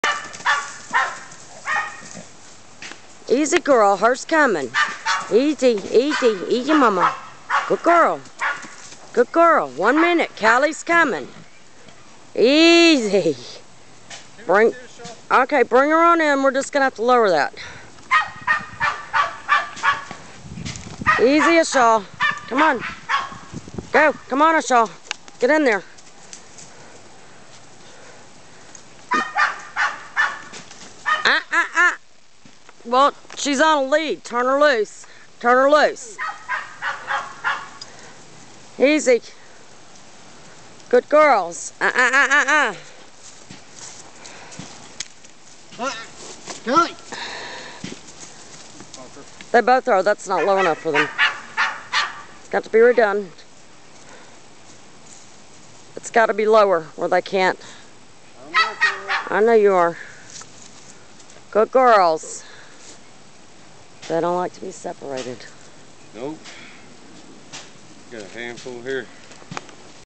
Asha Anxiety when seperated from Callie
Asha loves Callie August 14, 2010
Can't stand to be seperated from Callie not one minute